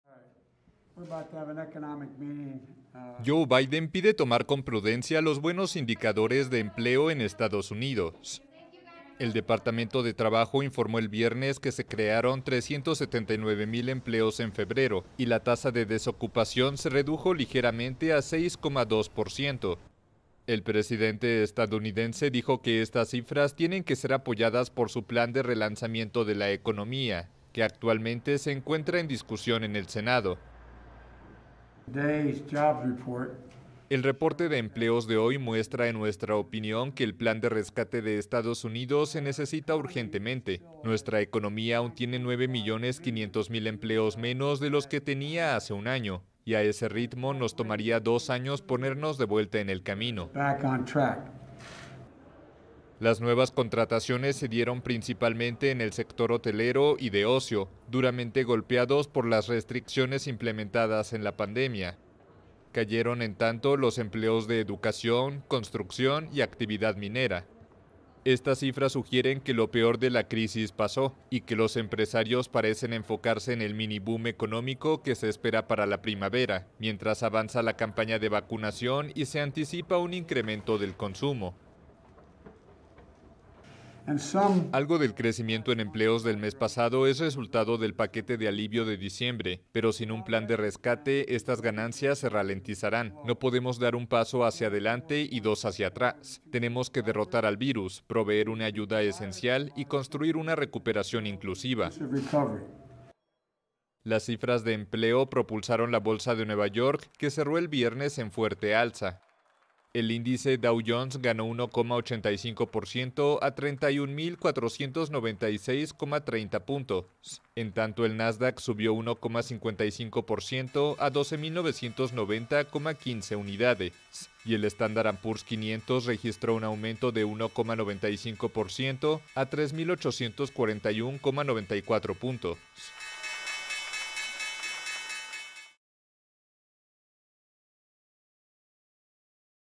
Biden modera optimismo de EEUU por buenas cifras del empleo | AFP
El presidente Joe Biden atemperó el entusiasmo generado por estimulantes cifras del empleo e insistió con la necesidad de adoptar su plan de relanzamiento de la economía de Estados Unidos.